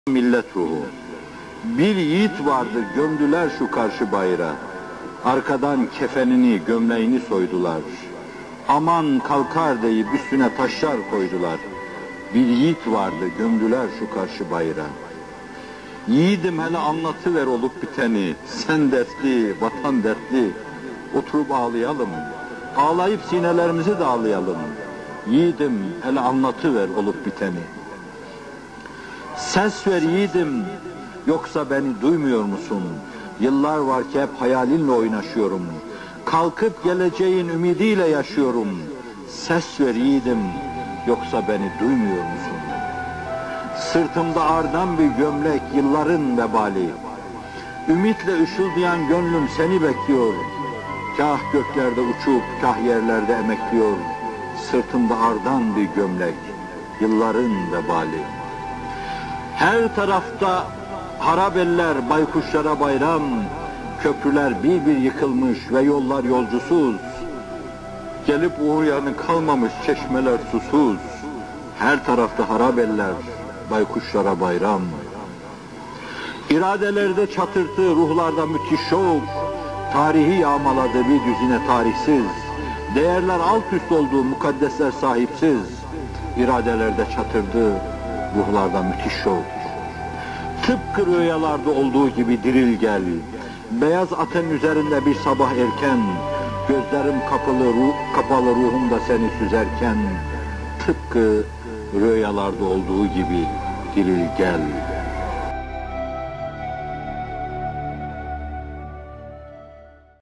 Ses Ver Yiğidim | M. Fethullah Gülen
Bir yiğit vardı gömdüler şu karşı bayıra
Arkadan gömleğini, kefenini soydular
Aman kalkar deyip üstüne taşlar koydular
Yiğidim hele anlatıver olup biteni
Sen dertli, vatan dertli, oturup ağlayalım
Ağlayıp da sinelerimizi dağlayalım

Ses ver yiğidim, yoksa beni duymuyor musun
Yıllar var ki hayalinle hep oynaşıyorum
Kalkıp geleceğin ümidiyle yaşıyorum

Sırtımda ardan bir gömlek, yılların vebali
Ümitle ışıldayan gönlüm seni bekliyor
Kah göklerde uçup, kah yerlerde emekliyor

Her tarafta harap eller, baykuşlara bayram
Köprüler bir bir yıkılmış ve yollar yolcusuz
Gelip uğrayanı kalmamış, çeşmeler susuz

Tıpkı rüyalarda olduğu gibi dirl gel
Beyaz atının üzerinde bir sabah erken
Gözlerim kapalı, ruhumda seni süzerken
Tıpkı rüyalarda olduğu gibi diril gel

Fethullah Gülen